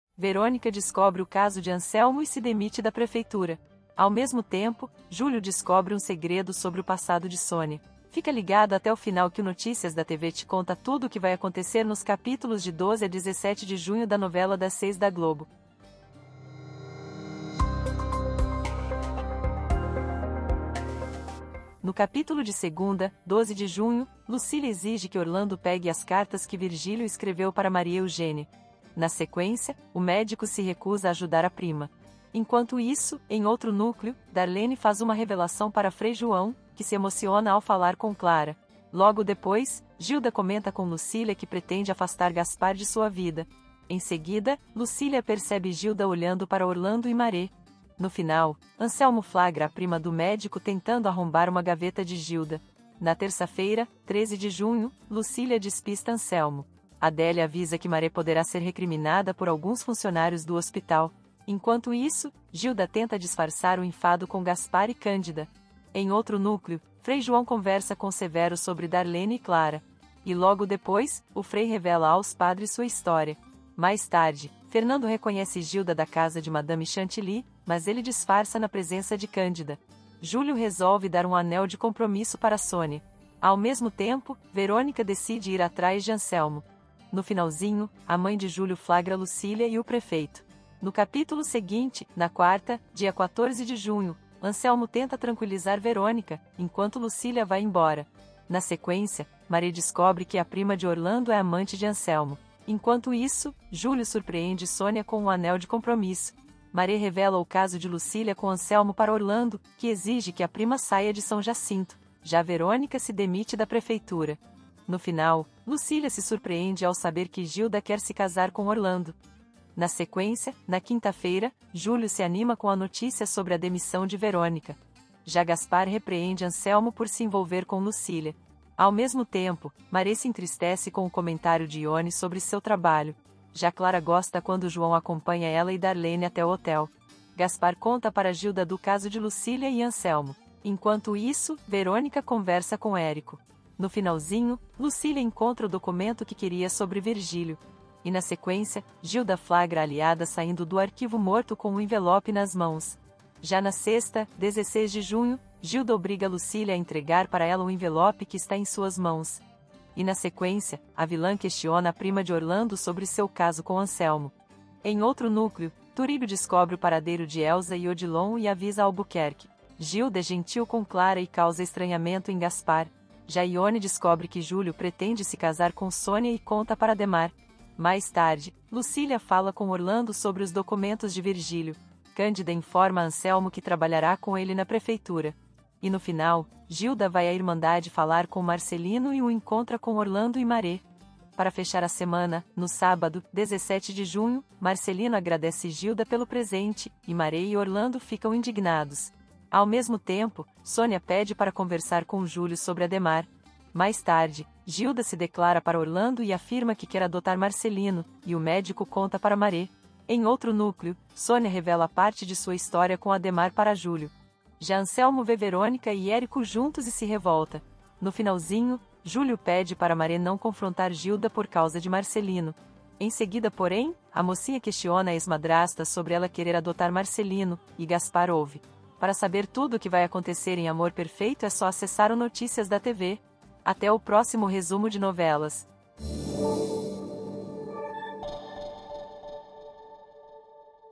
AMOR PERFEITO: RESUMO SEMANAL COMPLETO DE 12 A 17 DE JUNHO DE 2023
Verônica descobre o caso de Anselmo e se demite da prefeitura! Ao mesmo tempo, Júlio descobre um segredo sobre o passado de Sônia. Fica ligado até o final que o Notícias da TV te conta tudo o que vai acontecer nos capítulos de 12 a 17 de junho.

Acesse o Notícias da TV e acompanhe o resumo da novela e o que vai acontecer nos próximos capítulos de AmorPerfeito, da Globo